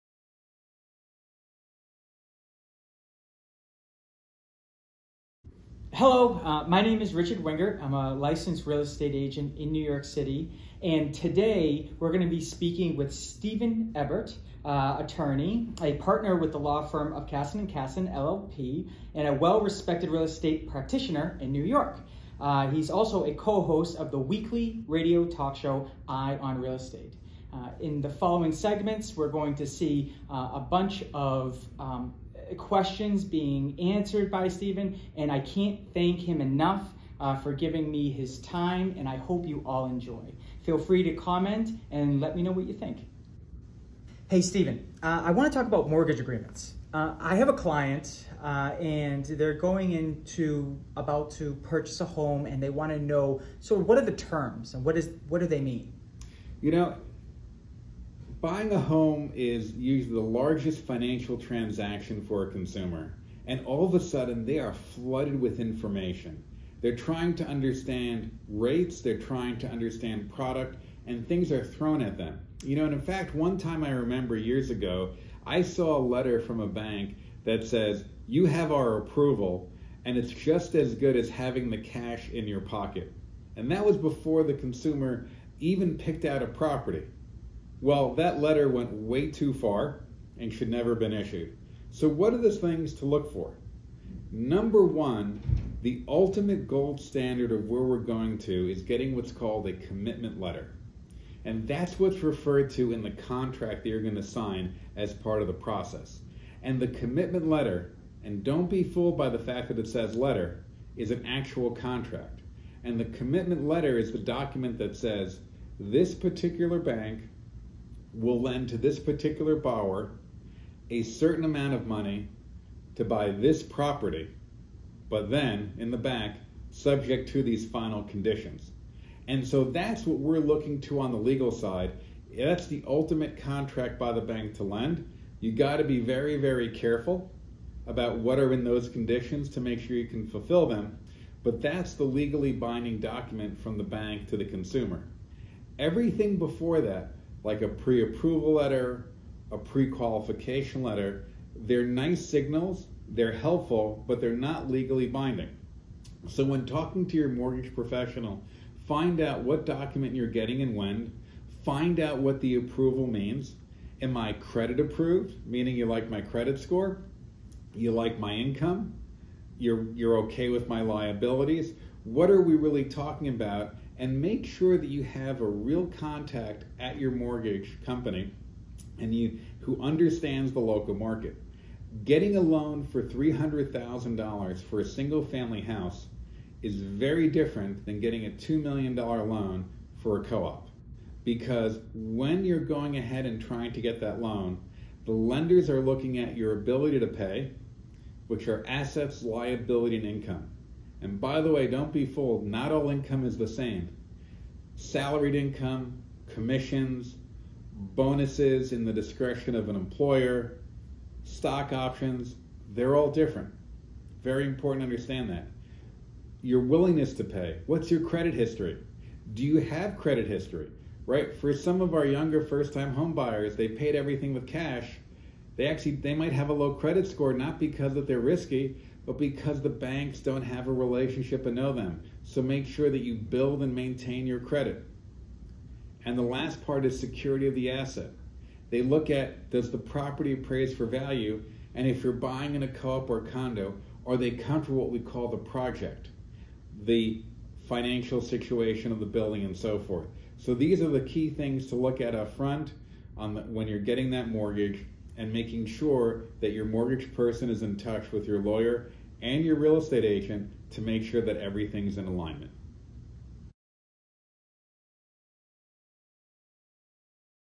Richard Wingert - Steven Ebert - Mortgage Agreement
The term "Mortgage Agreement" in the context of buying a home in New York City typically refers to the legal document that outlines the terms and conditions of the mortgage loan between the borrower (buyer) and the lender. This agreement is a crucial component of the homebuying process and establishes the rights and obligations of both parties. Here are key elements typically found in a Mortgage Agreement:

1) Loan Amount:
The Mortgage Agreement specifies the principal amount of the loan, which is the total amount borrowed by the buyer to purchase the property.

2) Interest Rate:
The agreement outlines the interest rate that the borrower will pay on the loan. The interest rate can be fixed or variable, depending on the terms of the mortgage.

3) Loan Term:
The loan term is the duration over which the borrower is required to repay the loan. Common terms include 15, 20, or 30 years.

4) Repayment Schedule:
The Mortgage Agreement details the schedule of payments, including the frequency (monthly, bi-weekly) and the amount of each installment.

5) Prepayment Penalties:
Some mortgage agreements include provisions for prepayment penalties, which are fees charged if the borrower pays off the loan before the scheduled term. Buyers should carefully review and understand any prepayment terms.

6) Late Payment Fees:
The agreement may specify the consequences of late payments, including any fees or penalties associated with missed deadlines.

7) Escrow Accounts:
Many mortgage agreements require borrowers to establish and contribute to escrow accounts for property taxes and homeowners insurance. The lender manages these accounts and pays these expenses on behalf of the borrower.

8) Conditions for Default:
The Mortgage Agreement outlines the conditions under which the borrower would be considered in default. Default conditions may include failure to make payments, violation of loan covenants, or other specified events.

9) Property Use:
The agreement may include clauses regarding the allowable use of the property. For example, if the property is purchased as a primary residence, there may be restrictions on renting it out.

10) Insurance Requirements:
The borrower is typically required to maintain adequate homeowners insurance on the property, and the agreement may outline the insurance requirements.

11) Security Interest:
The Mortgage Agreement establishes the lender's security interest in the property. In the event of default, the lender may have the right to foreclose on the property to recover the outstanding loan amount.

12) Transfer of Ownership (Due-on-Sale Clause):
Some mortgage agreements include a due-on-sale clause, which gives the lender the right to accelerate the loan if the property is sold or transferred to a new owner.

The Mortgage Agreement is a legally binding document, and both parties should carefully review and understand its terms before signing. It's advisable for buyers to work with a qualified real estate attorney to ensure that the agreement aligns with their interests and that they fully comprehend the financial implications of the mortgage loan.